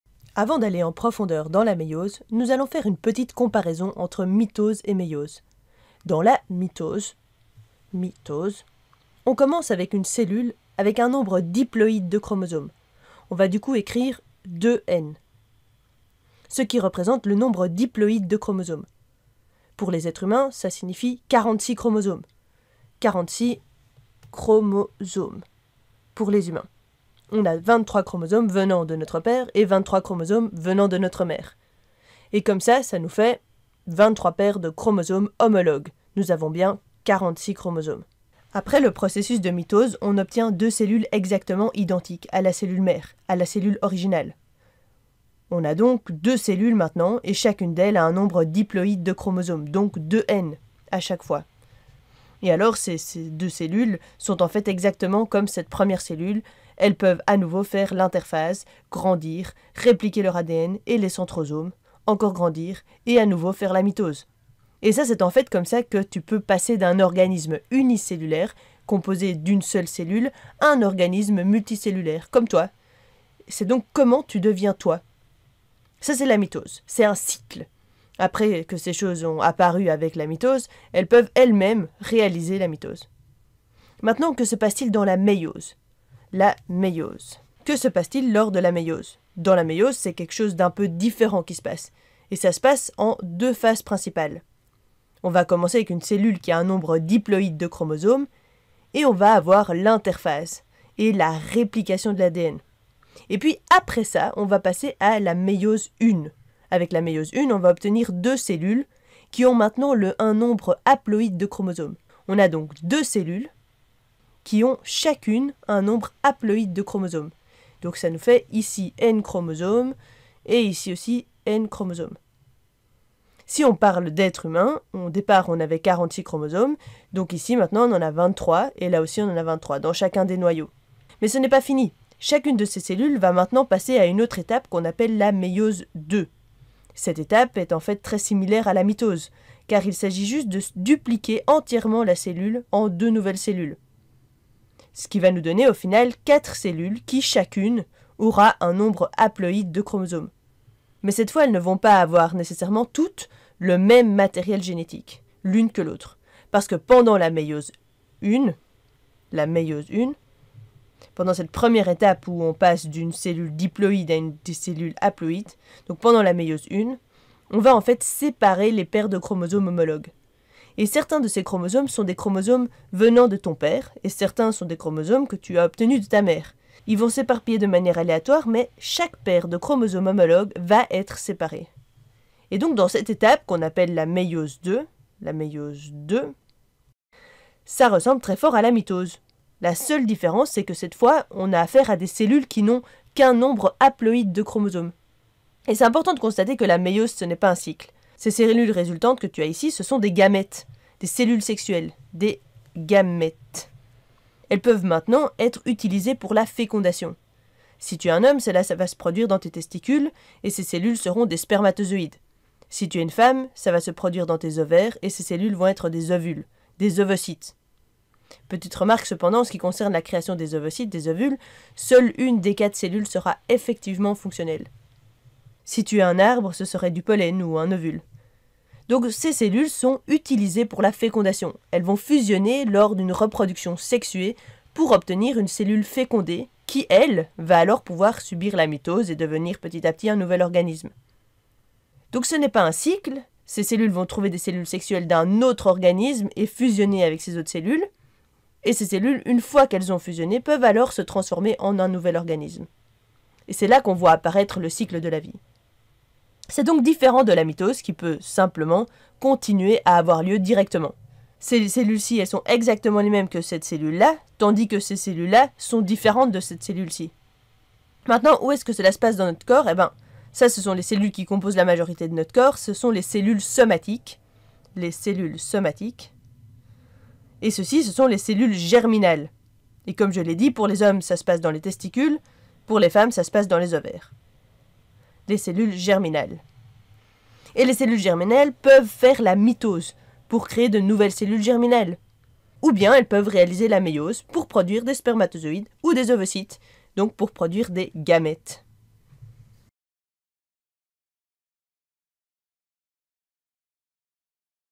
Comparaison entre mitose et méiose
Découvrez dans cette vidéo les éléments qui différencient les processus de mitose et de méiose qui sont tous deux des processus de division cellulaire.